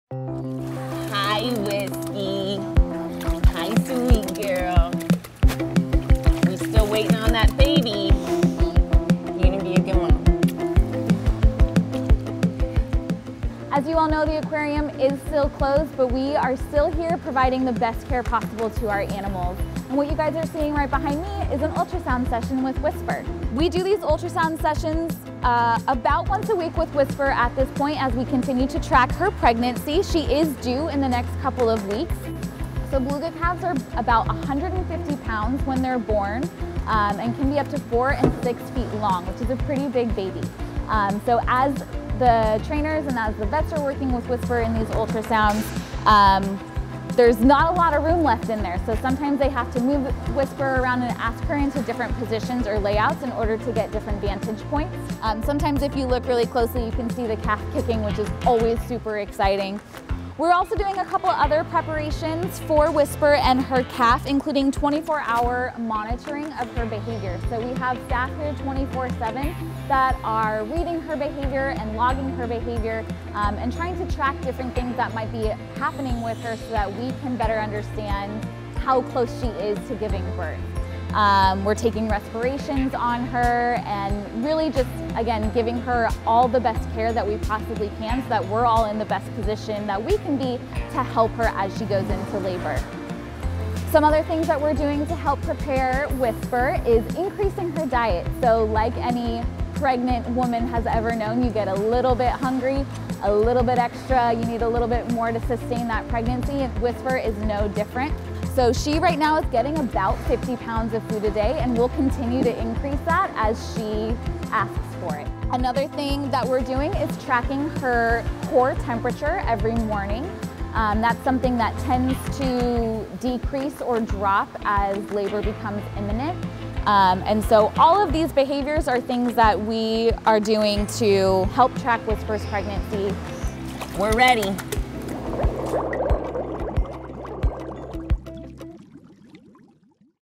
Ultrasound with Pregnant Beluga Whisper!
Beluga whale bump-date: Pregnant whale Whisper is due in the coming weeks! 🥰 Even though we're closed, Whisper, and all of our animals, still continue to receive the best care possible from our amazing teams. Whisper gets weekly ultrasounds, her diet is increasing as she and the calf continue to grow, and she’s now under 24-hour behavior monitoring. We’re looking forward to what’s ahead and will provide updates on our channels!

Watch the video and see her latest ultrasound check-in with our animal care and veterinary teams!